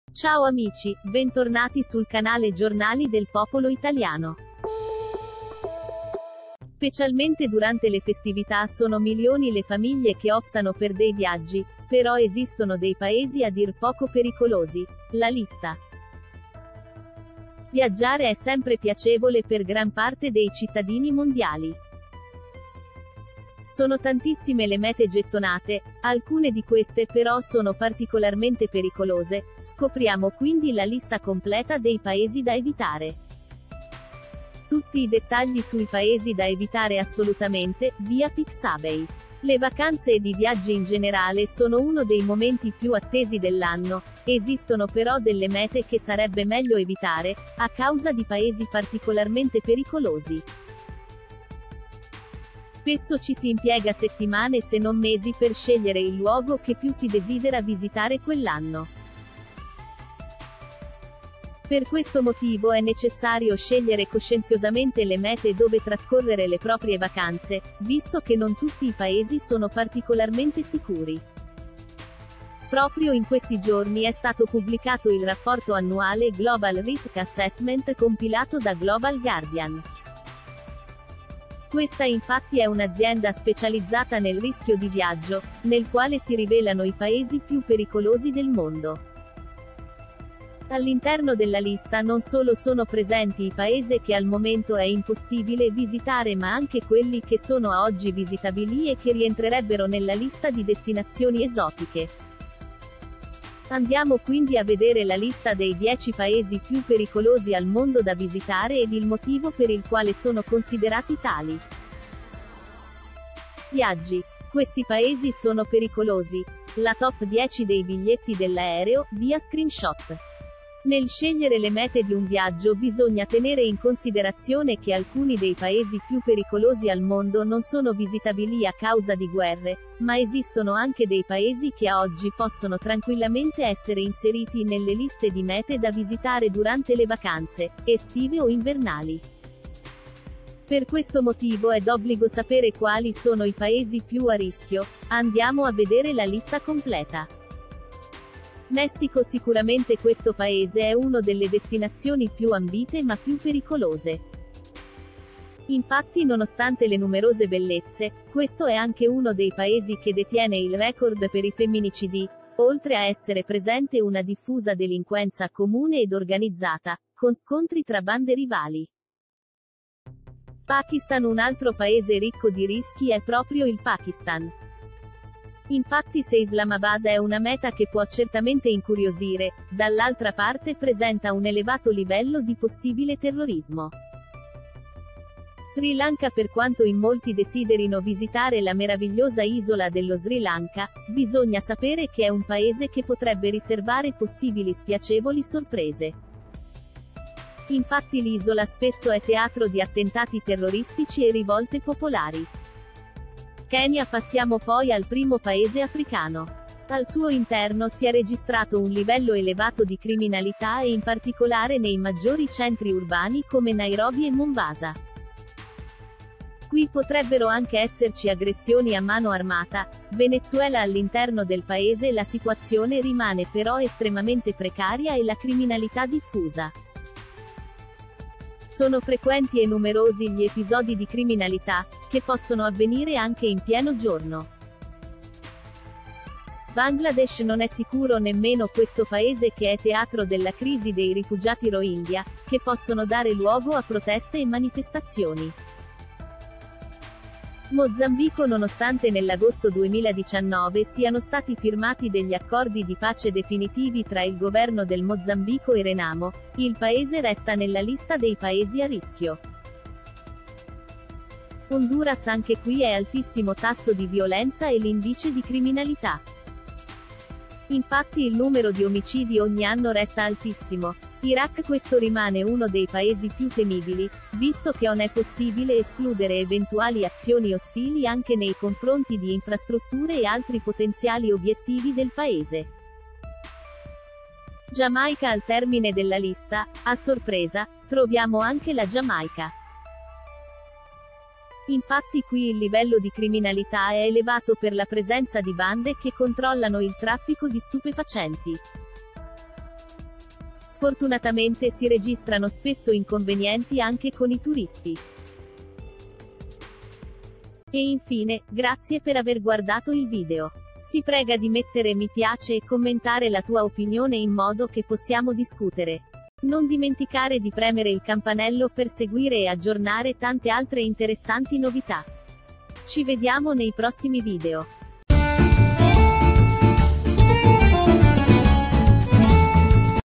Viaggi, meglio non andare in vacanza qui: la lista dei paesi più pericolosi - Inews24.it
Specialmente durante le festività sono milioni le famiglie che optano per dei viaggi, però esistono dei paesi a dir poco pericolosi: la lista.  Viaggiare è sempre piacevole per gran parte dei cittadini mondiali. Sono tantissime le mete gettonate, alcune di queste però sono particolarmente pericolose: scopriamo quindi la lista completa dei paesi da evitare.  Tutti i dettagli sui paesi da evitare assolutamente (Via Pixabay) Le vacanze ed i viaggi in generale sono uno dei momenti più attesi dell’a